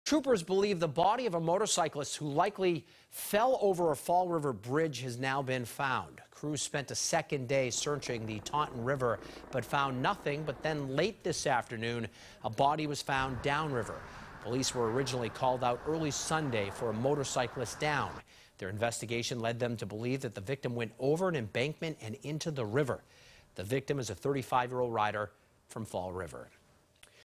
Police Believe They Found Body Of Missing Motorcyclist In Taunton River
Troopers believe they found the body of a motorcyclist who likely went over a Fall River bridge.